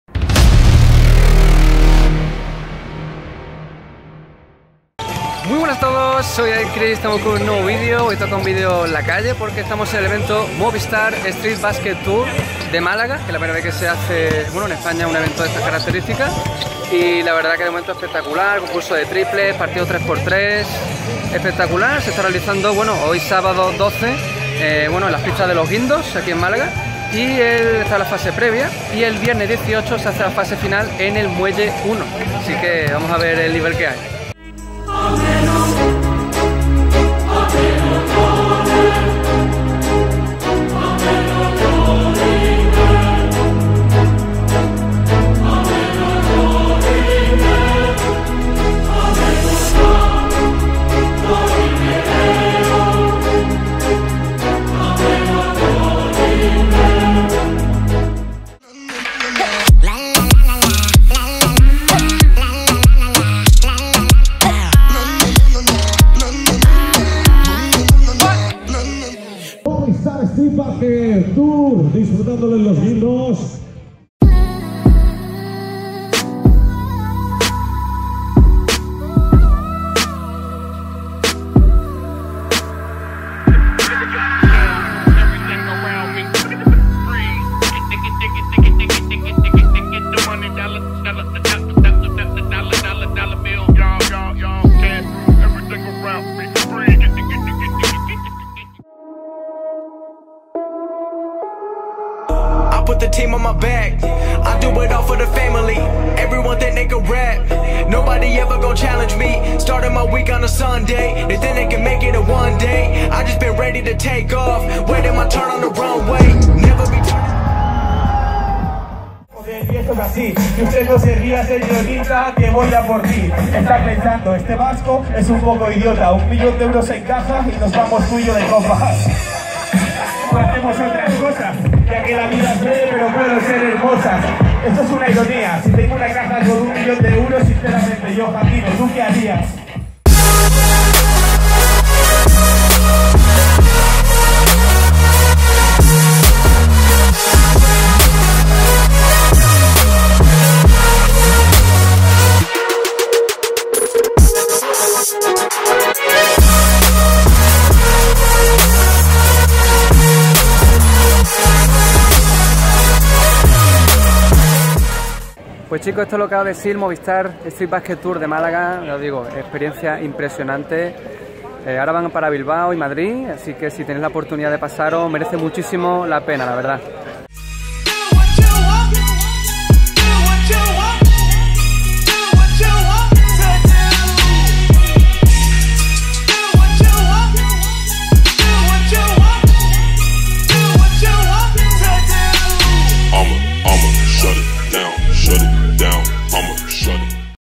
¡INCREÍBLE! Movistar Street Basket Tour Málaga - VLOG AIRCRISS
Aquí tenéis un resumen de la experiencia vivida en Málaga con la fase previa y final del Movistar Street Basket Tour de la ACB donde estuvieron presentes también jugadores del Unicaja.
► ¿Cómo grabo las partidas? Con

Listado de canciones que uso y Epidemic Sounds